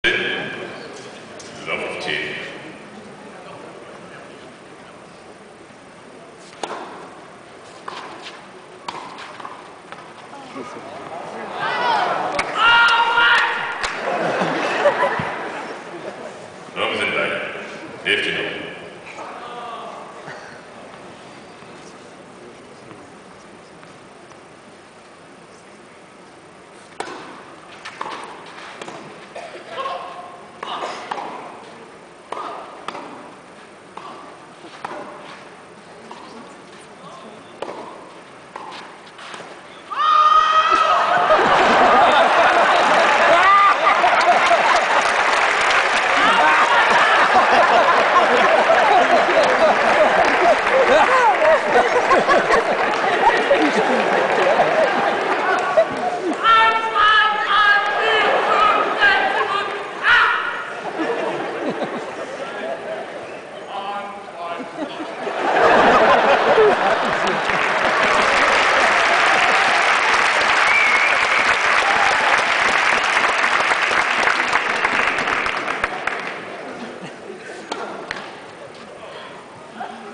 Henri Leconte being funny at Porsche Tennis Grand Prix 2011
Henri Leconte played a mixed doubles exhibition match at the Porsche Tennis Grand Prix 2011 in Stuttgart. He played with Rittner against Lisicki and Steeb.